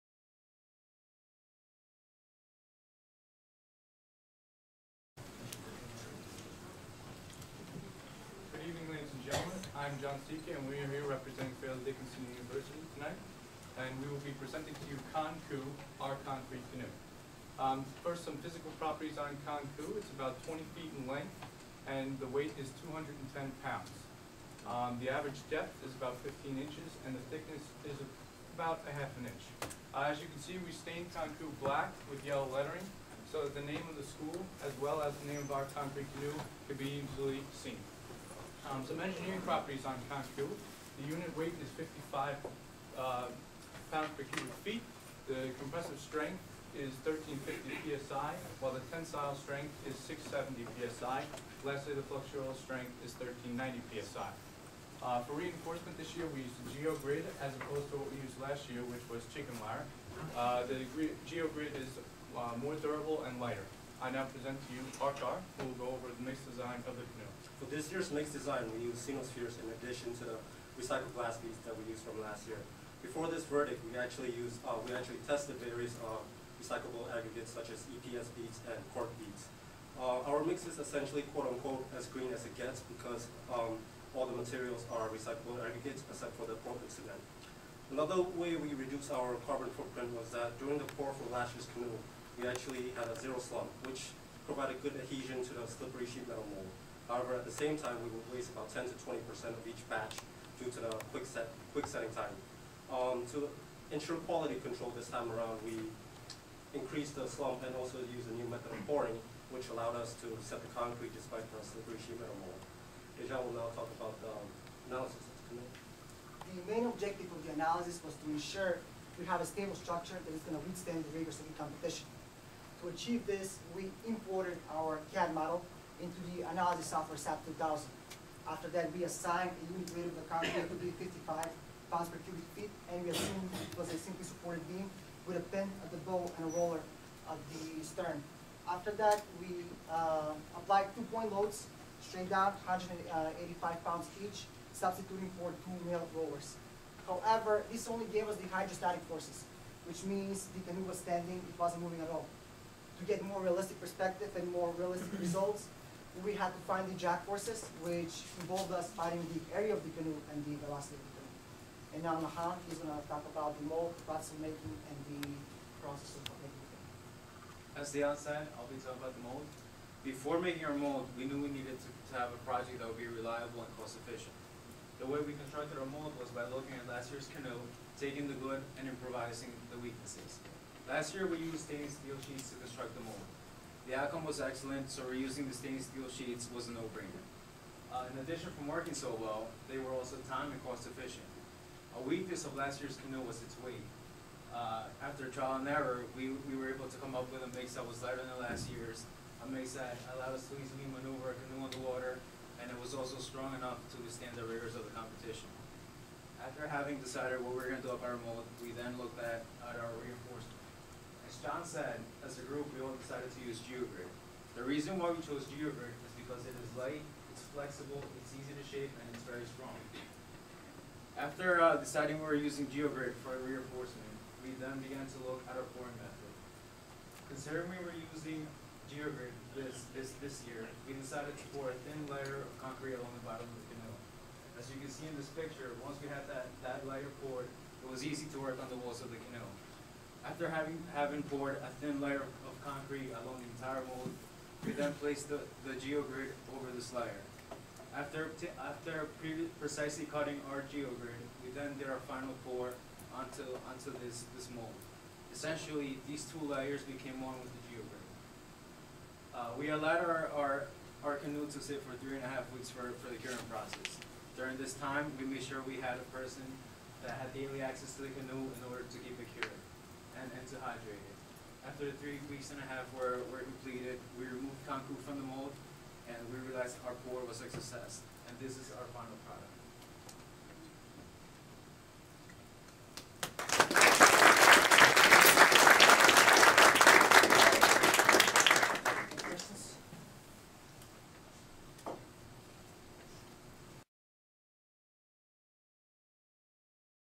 ASCE 2010 Metropolitan Conference FDU's Presentation.wmv
2010 ASCE Metropolitan Area Conference - Presentation Night

FDU's Presenters:  John Sica, Arkar Lwin, Dejan Donev, Nahon Torres